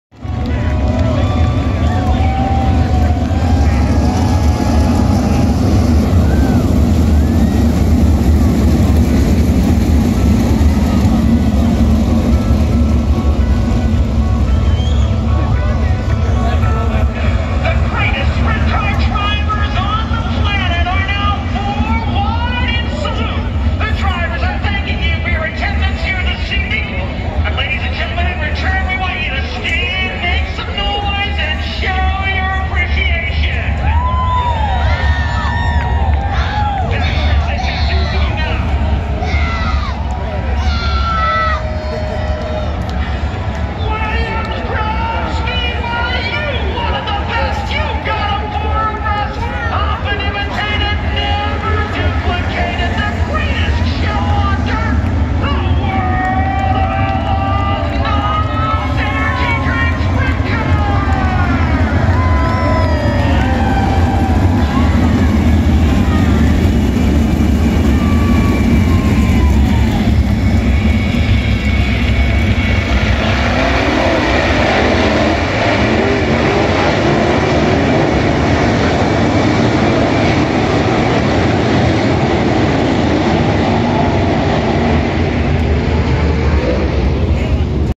World of Outlaws Sprint car 4 wide salute to the fans, Williams Grove PA Speedway 2020 intro
WoO nos energy drink sprint cars Pennsylvania posse,